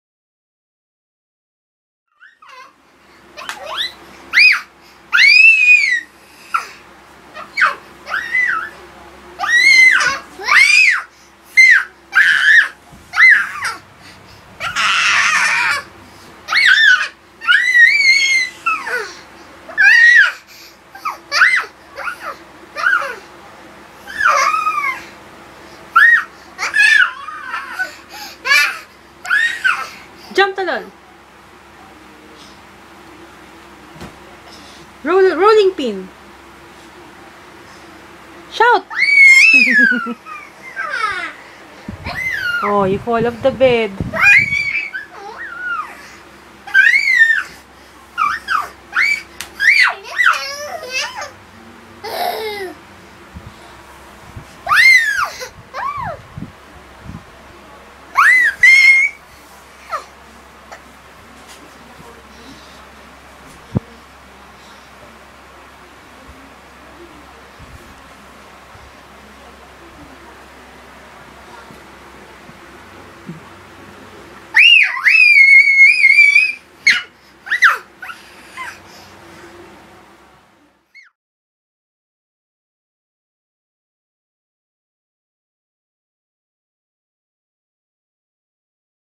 Rolling in the Bed - Playtime with Zac
One of Zac's playtime favorites! Jumping and rolling on the bed!